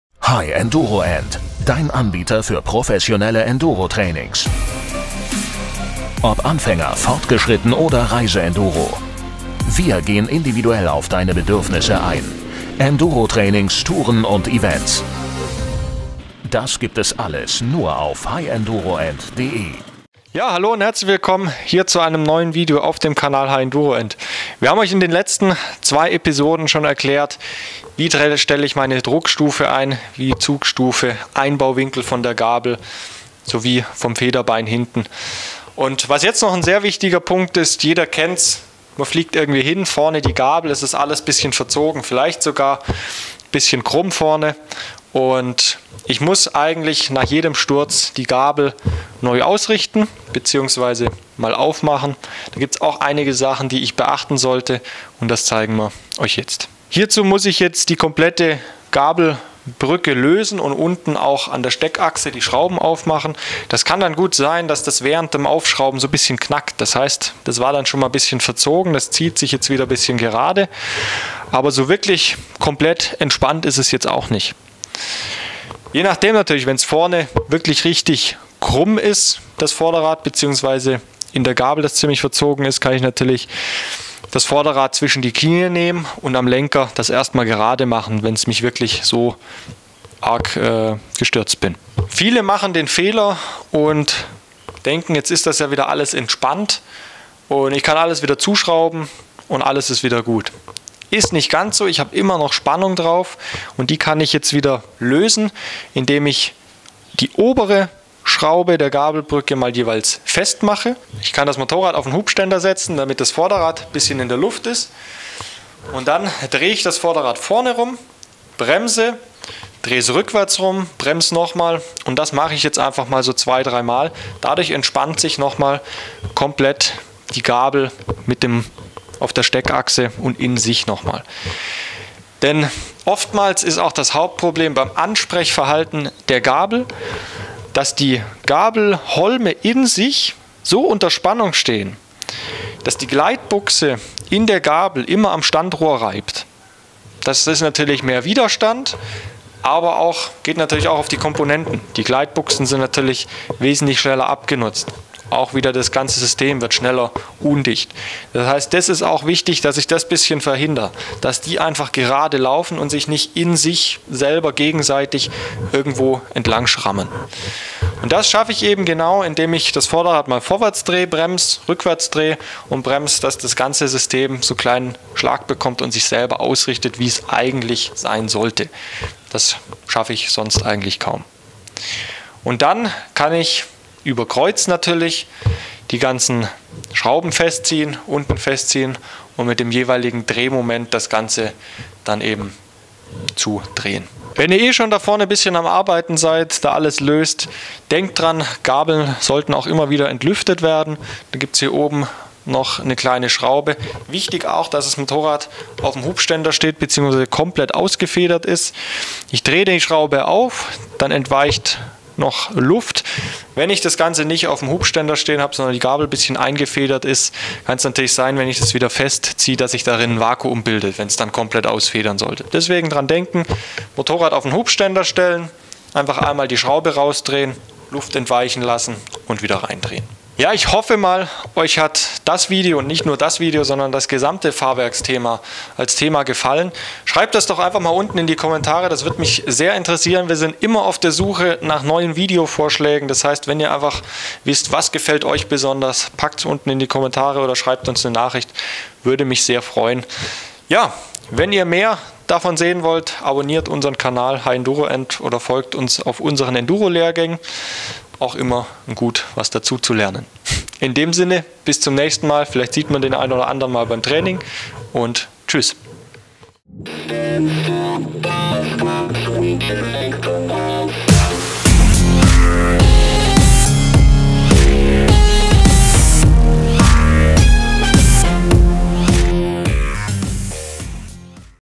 Enduro Tutorial #21 / Fahrwerk Teil 3 / Gabel & Vorderrad ausrichten
Unser Fahrwerk der Enduro ist ein sehr wichtiges und kompliziertes Bauteil. Umso wichtiger ist es, dass ich bescheid weiß wie sich alles genauestens verhält. Ihr wisst nicht wie? Wir zeigen euch die wichtigsten Tipps und Tricks.

Moderation:
Leon Busse, HighEnduroEnd

Kamera:
Nikon D800